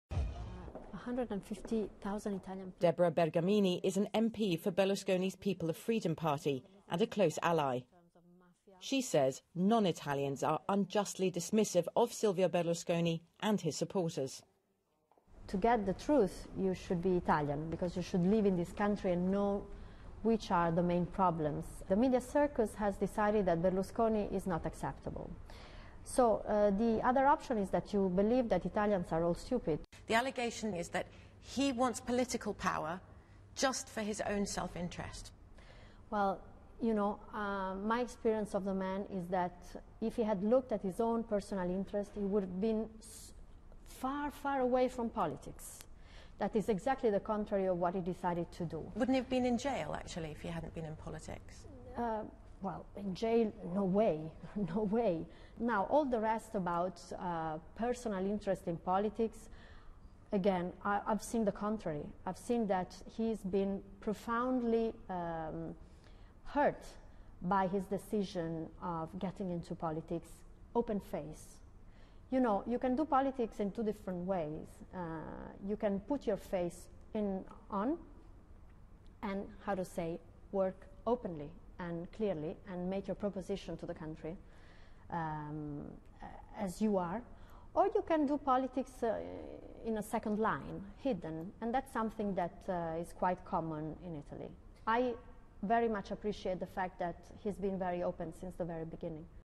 Deborah Bergamini spiega il 'fenomeno Berlusconi' alla BBC
All'estero Berlusconi e i suoi elettori sono guardati ingiustamente dall'alto in basso. Questo perché i media hanno deciso che Berlusconi è inaccettabile, ma credere ciò significherebbe credere che gli italiani sono stupidi.

Se Berlusconi avesse guardato al suo interesse personale se ne sarebbe stato lontano anni luce dalla politica.
La sua decisione di scendere in campo gli ha procurato molti attacchi.

Si può fare politica in due modi: apertamente, facendo una proposta chiara al paese, o nascosti in seconda linea, dietro le quinte, cosa molto comune in Italia. Ho molto apprezzato che lui ci abbia messo la faccia fin dall'inizio.